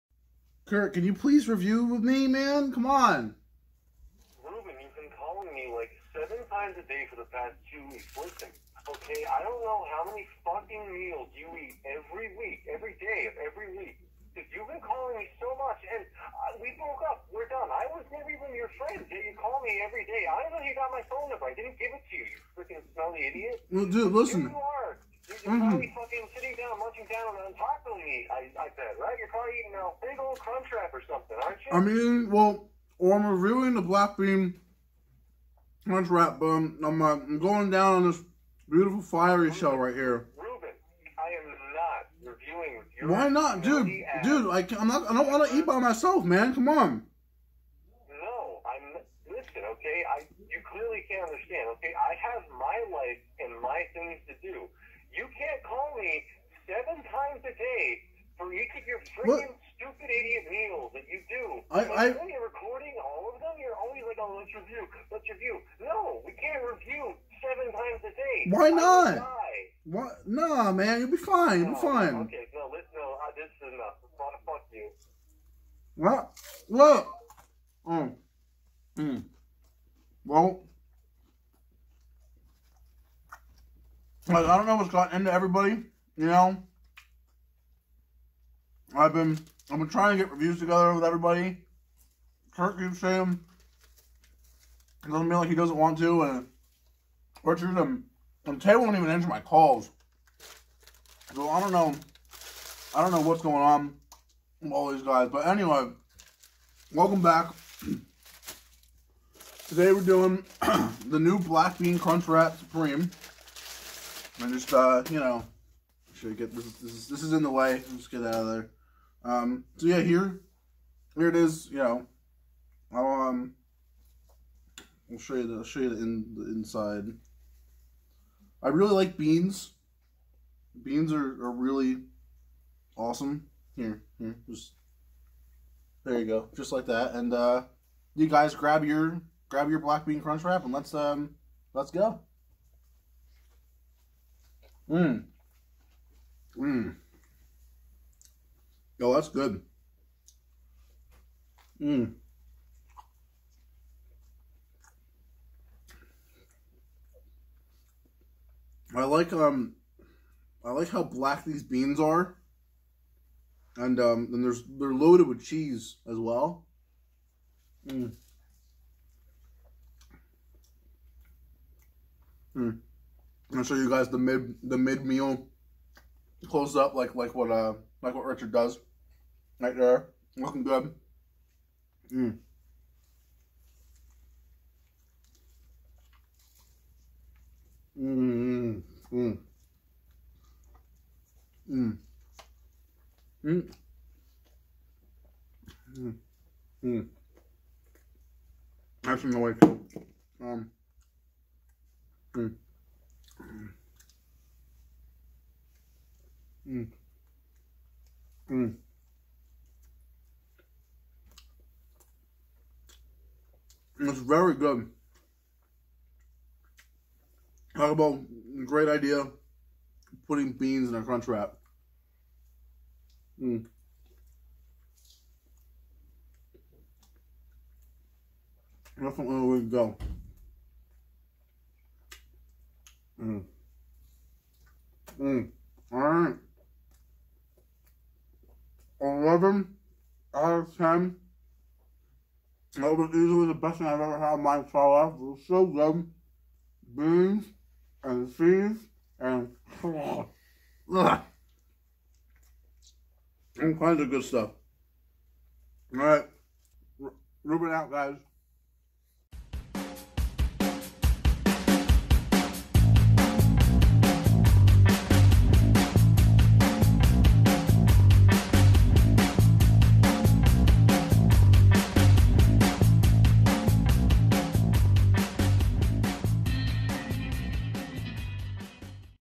reuben eats taco bell's NEW black bean crunchwrap
that tasted pretty good